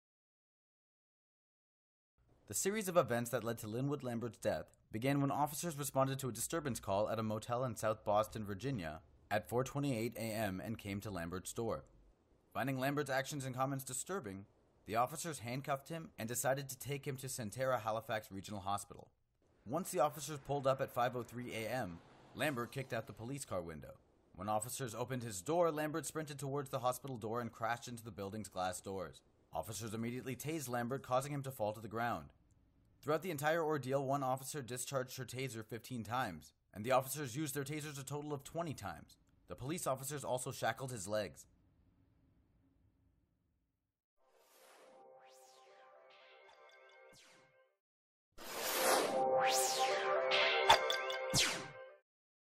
Videos show Virginia police using Tasers on man who died in custody
Originally published on 12 November, 2015

Linwood Lambert, a 46-year-old construction worker, was only supposed to go to the ER for medical attention on May 4, 2013, but he ended up dying in police custody. No charges were filed. Over two years later, the case is receiving new attention after MSNBC aired a report on Wednesday along with police and surveillance videos.

The series of events that led to Lambert’s death began when officers responded to a disturbance call at a motel in South Boston, Virginia at 4:28 a.m. and came to Lambert’s door. Finding Lambert’s actions and comments disturbing, the officers handcuffed him and decided to take him to Sentara Halifax Regional Hospital, reports Reuters.

Once the officers pulled up at the hospital at 5:03 a.m., Lambert kicked out the police car window. When officers opened his door, Lambert sprinted towards the hospital door and crashed into the building’s glass doors.

Officers immediately tased Lambert, causing him to fall to the ground. Officers then shackled his legs. Throughout the entire ordeal, the officers used their Tasers a total of 20 times. One officer discharged her Taser 15 times.

MSNBC — using figures obtained from company device reports — found that the three officers discharged their Tasers a total of 20 times in roughly half an hour, which MSNBC says amounts to approximately 87 seconds of tasing. This, when a single, 5-second Taser discharge generally incapacitates a person.

Police then decided to bring Lambert to jail. By the time the officers arrived at the jail, Lambert was unresponsive. He was later pronounced dead at Sentara Halifax Regional Hospital at 6:23 a.m.

According to MSNBC, Lambert’s autopsy lists “acute cocaine intoxication” as the cause of his death. His family disagrees with the autopsy report. They have filed a civil suit against the police alleging that excessive force and denial of medical care was the cause of Lambert’s death.